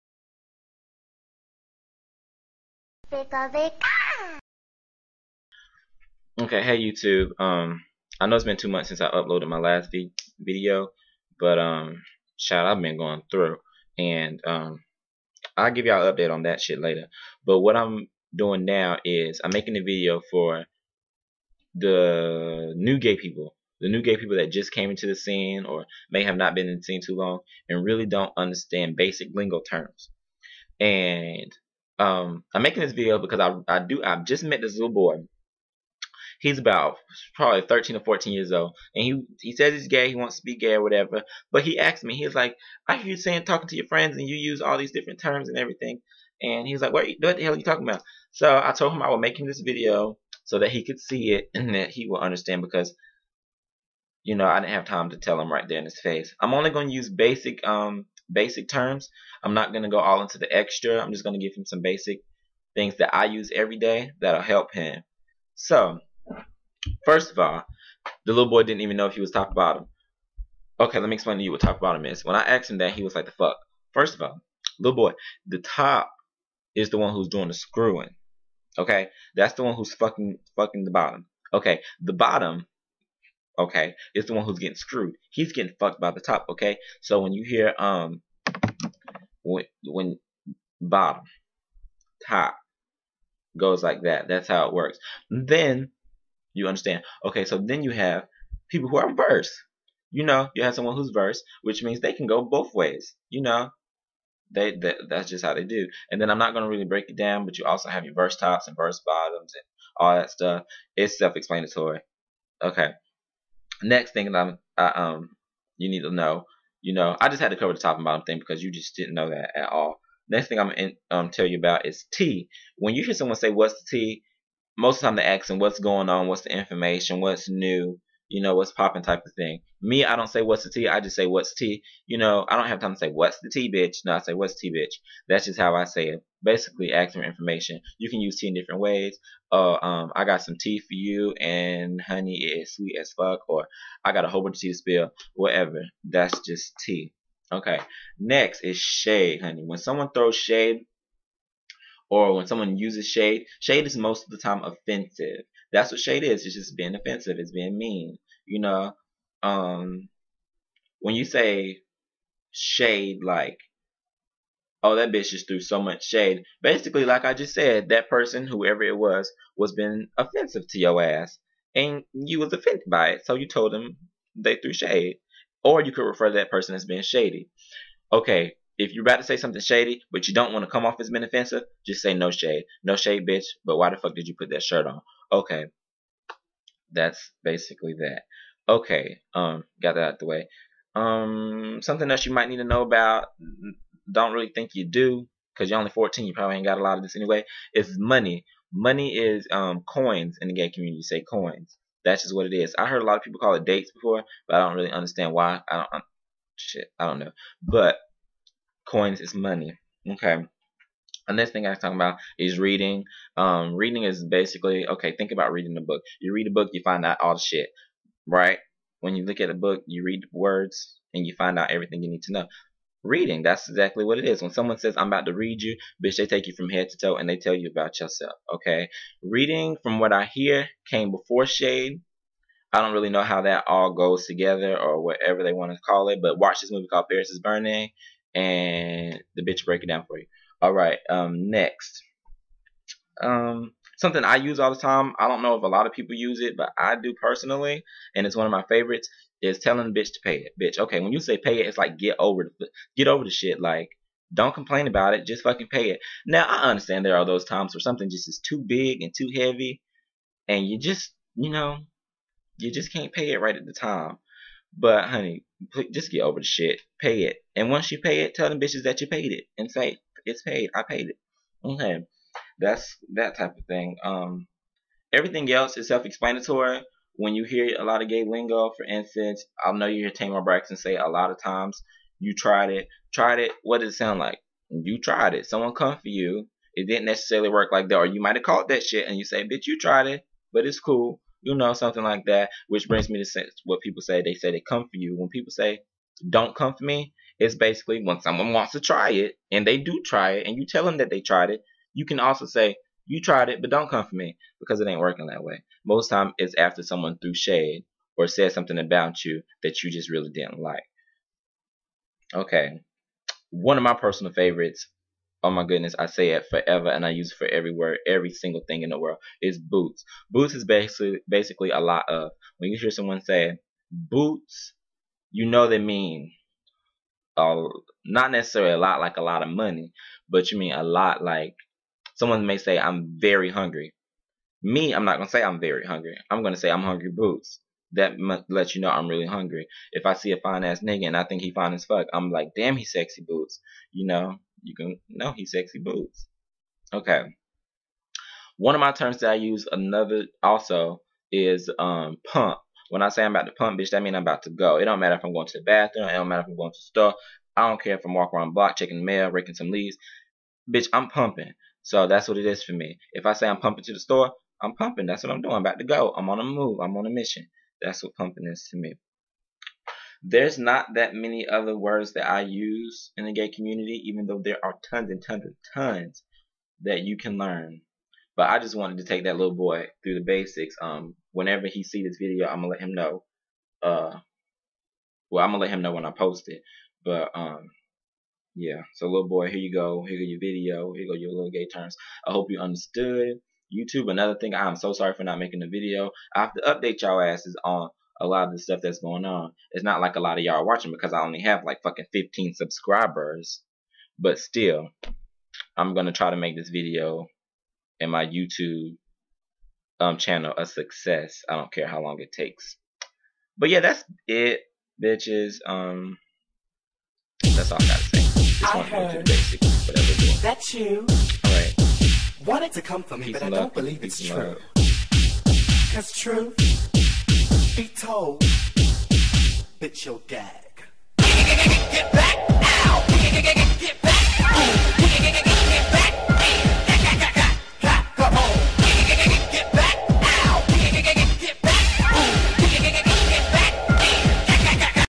Video Diary: My Gay Lingo Terms for the Little Boy
I made this for a little boy who walked up to me in the mall like a week ago.. I didn't forget about you baby lol.. :-)